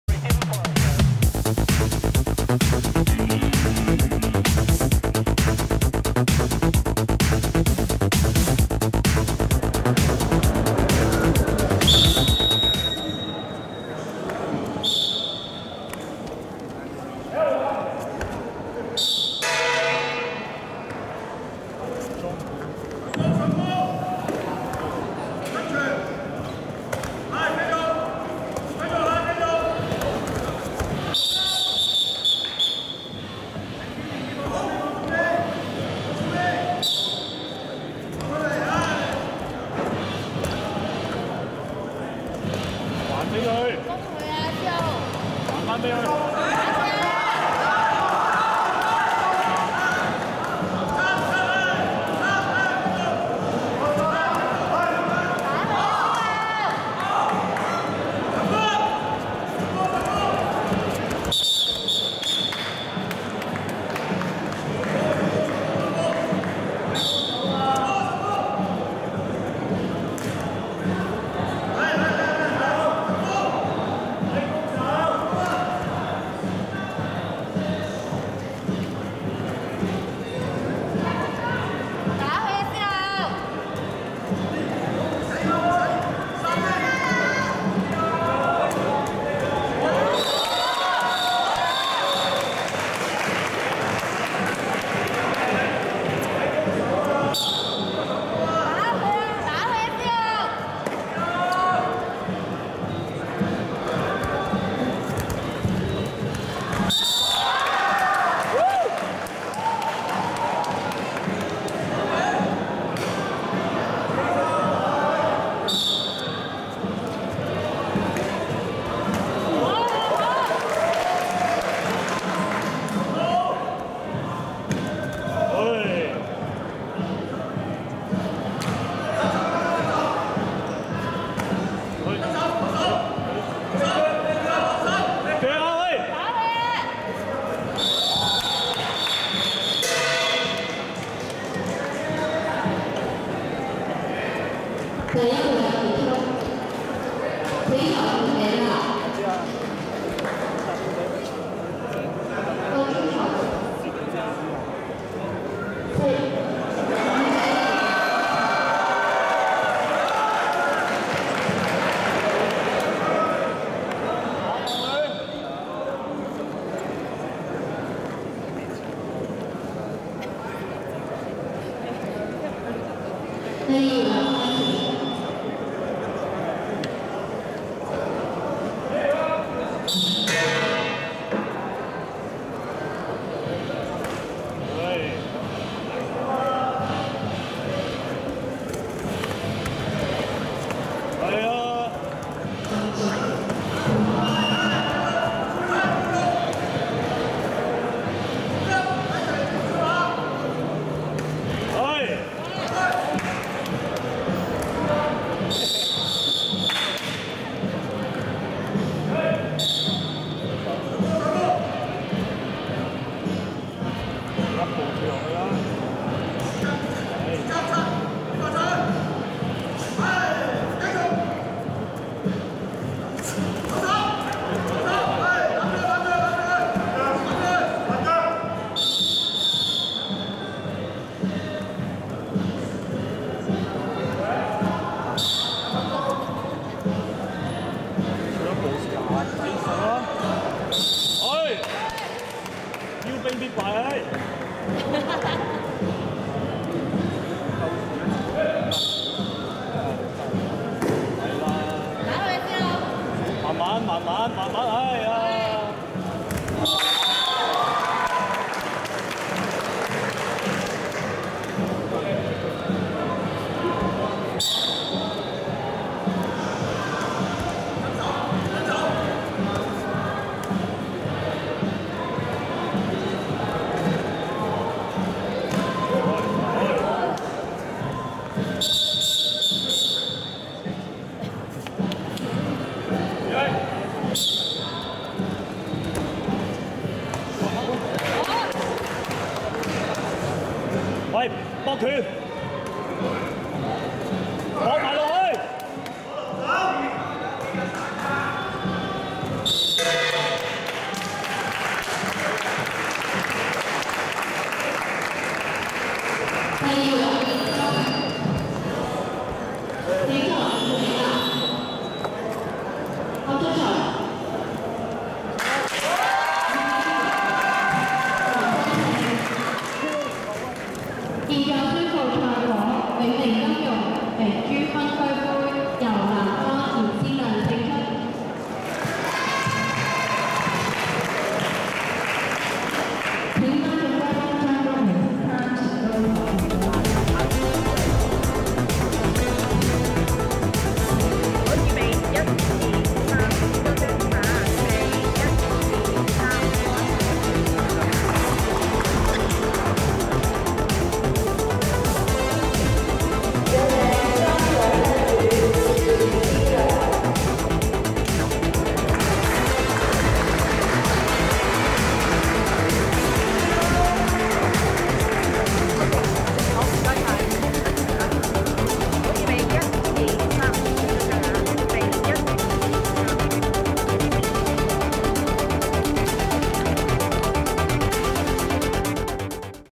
2014全港國術擂台邀請賽。第14場。永明金融明珠分區盃（2014 HONG KONG MARTIAL ARTS INVITATIONAL TOURNAMENT。FIGHT 14）
地點 Place: 2014/10/25 Hong Kong

第14場（FIGHT 14）
紅方     陳迪文  (莫劍華詠春拳術學會)
vs
藍方     饒思亮  (勵舘)